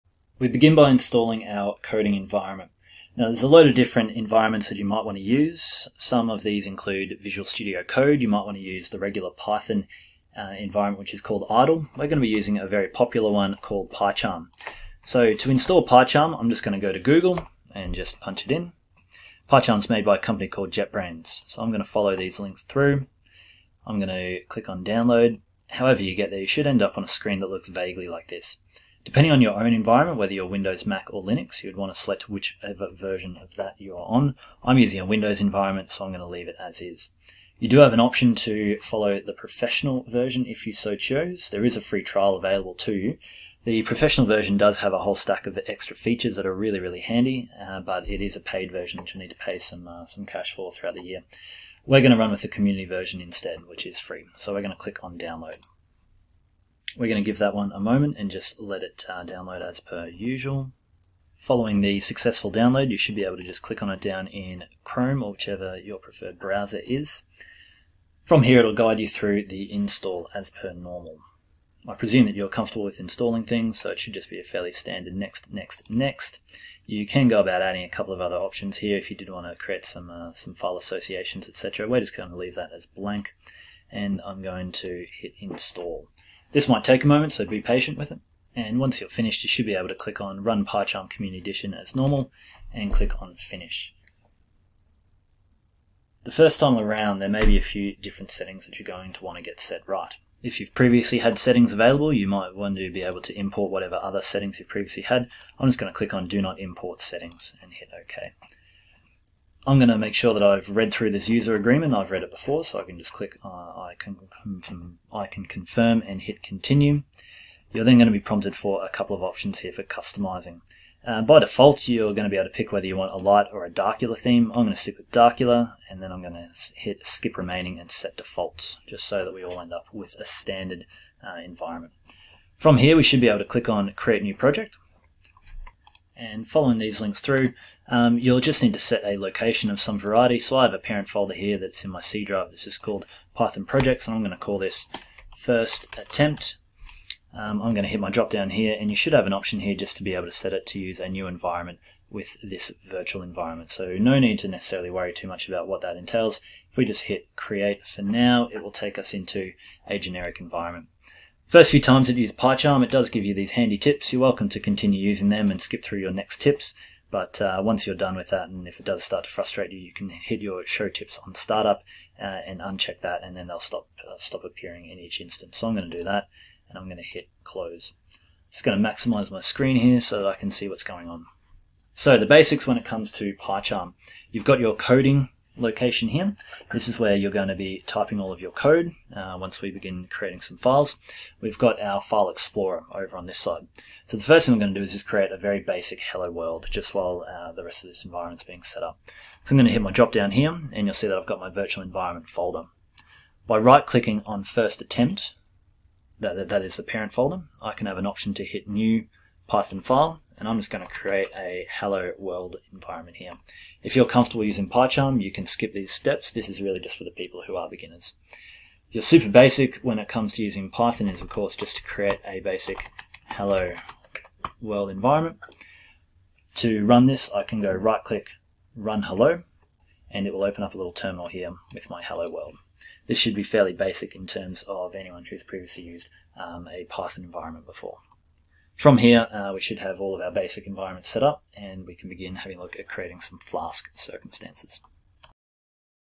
Python Flask Starting Out Tutorial - Installing PyCharm Community
How to set up your PyCharm coding environment.